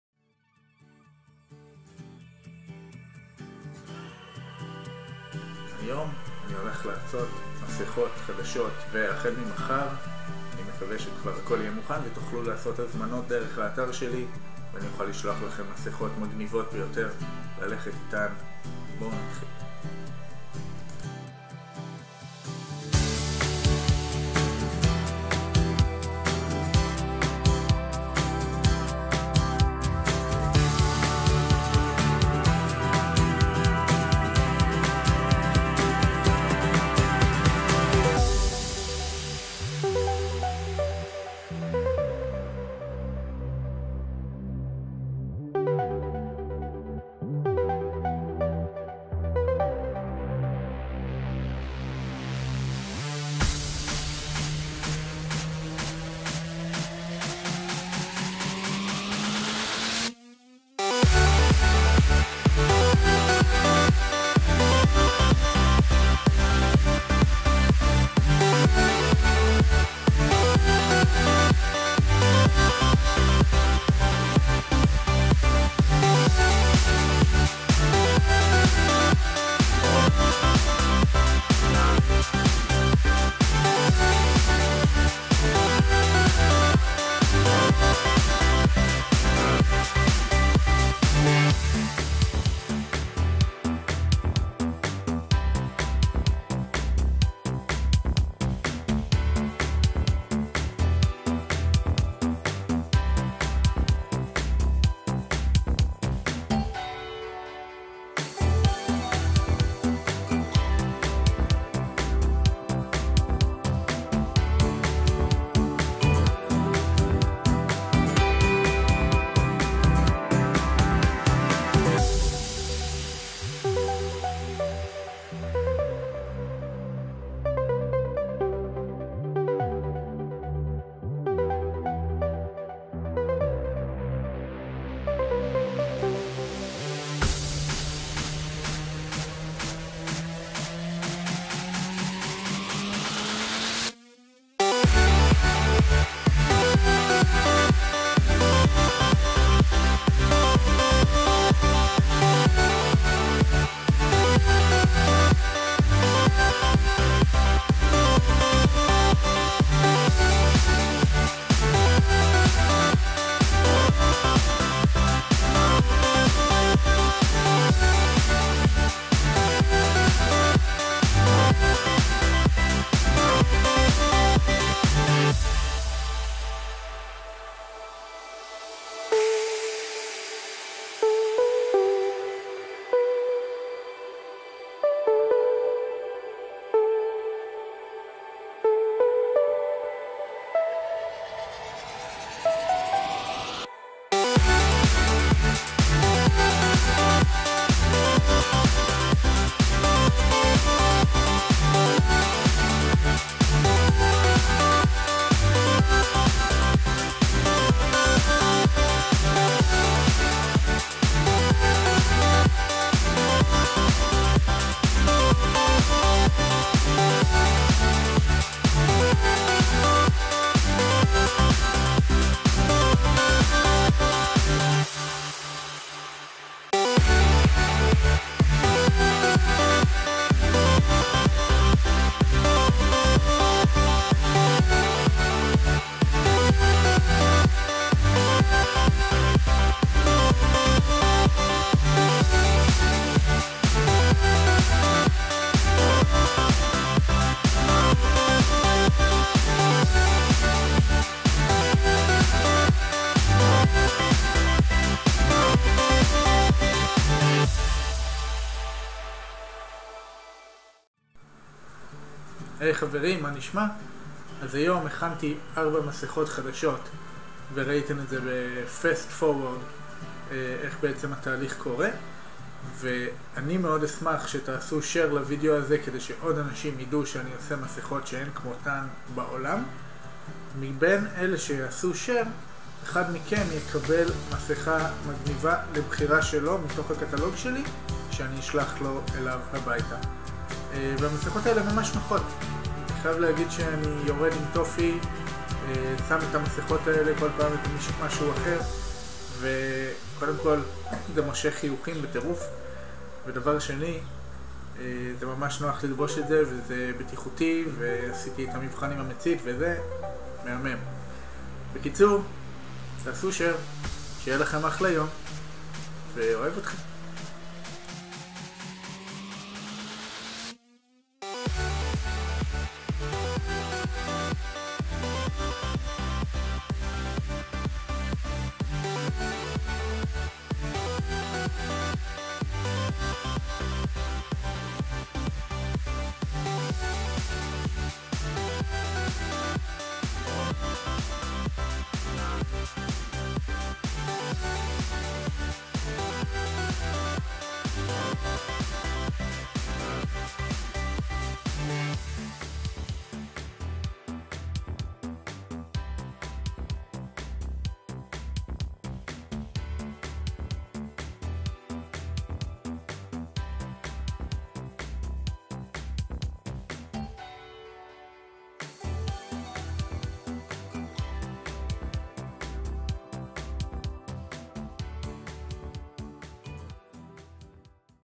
CUSTOM DESIGNED FACE MASKS
The situation with the covid19 (coronavirus) is such that it isn't going away anytime soon.
So, I have designed and created functional and awesome face masks, to help you cope with the situation.

Here's a look into the creation process.

My website for orders:
RoyiB.com